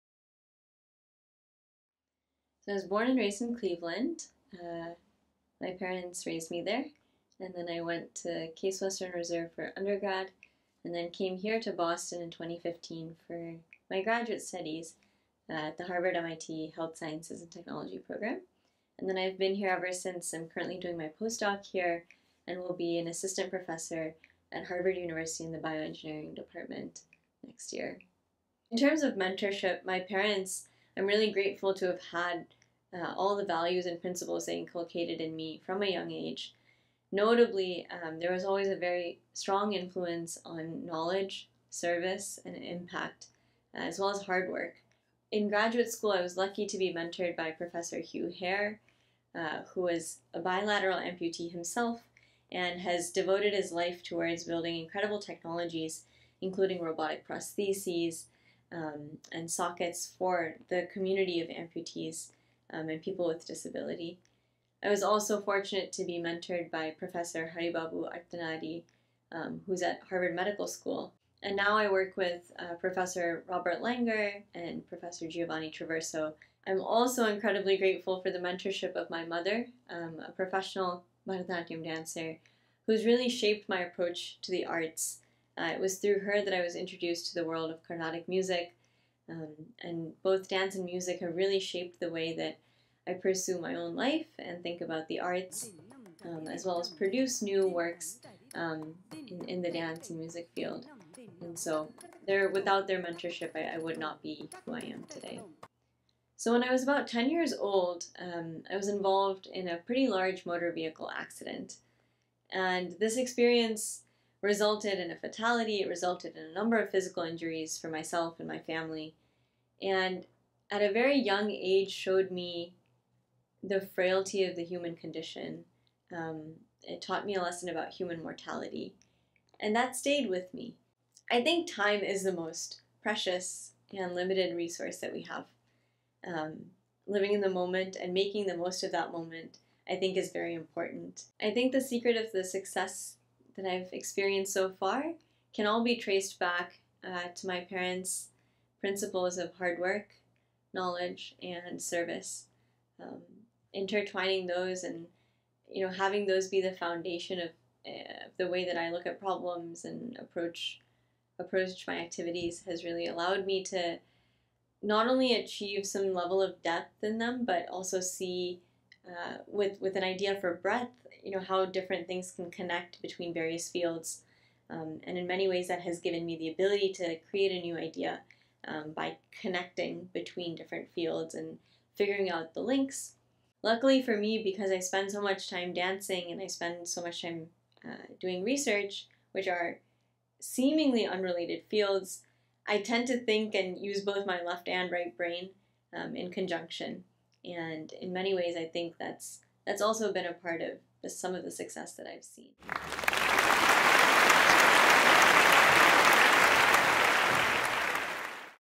When in Doubt, 2022 New England Choice Awards Winner Shriya Srinivasan Turns to Data First
When we first featured Shriya Srinivasan in INDIA New England News, it was in June 2017. Then, with other MIT researchers, she had devised a new surgical technique that could allow prosthetic limbs to feel much more like natural limbs.

Five years later, she is a Postdoctoral Researcher at Harvard Medical School, holds the distinction of being in the prestigious Forbes 30 Under 30 list, Director of Project Prana 501c3 NFP, and Co-Founder and Co-Director of Anubhava Dance Company. In addition, she is a Schmidt Science Fellow and Junior Fellow at the Harvard Society of Fellows.

On Nov. 19, she was bestowed with the Rising Star award at the annual New England Choice Awards 2022 at Hilton Woburn Hotel in Woburn, MA.